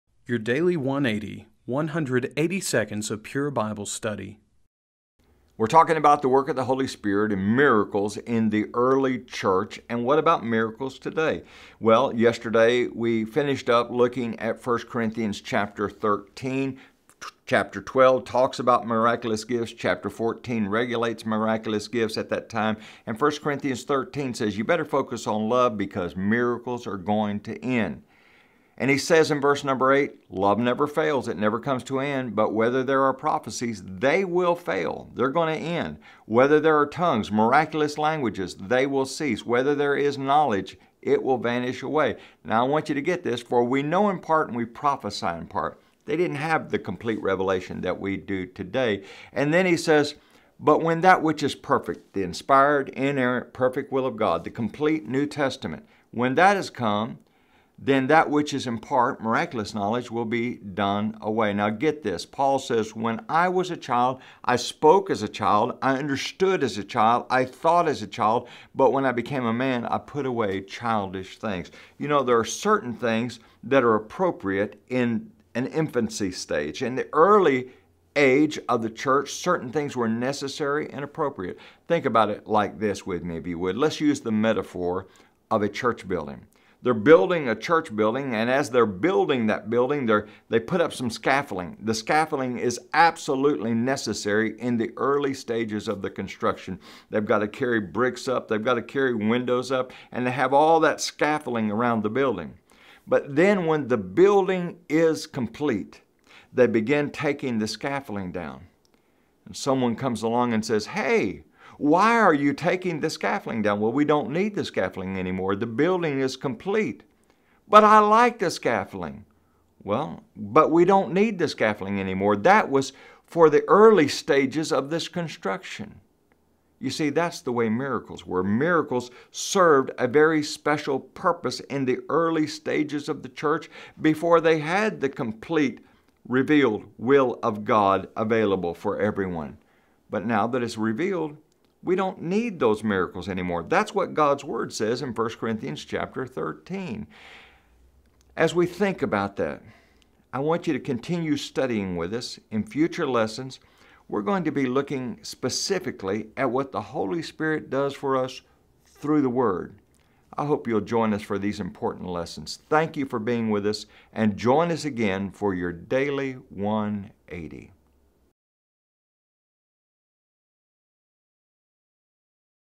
Your Daily 180: Friday, November 11th, 2022- The Holy Spirit: The Complete Revelation Stands Alone
The Holy Spirit has revealed the will of The Father through the apostles and prophets. Now that it is complete, we have all things pertaining to life and godliness.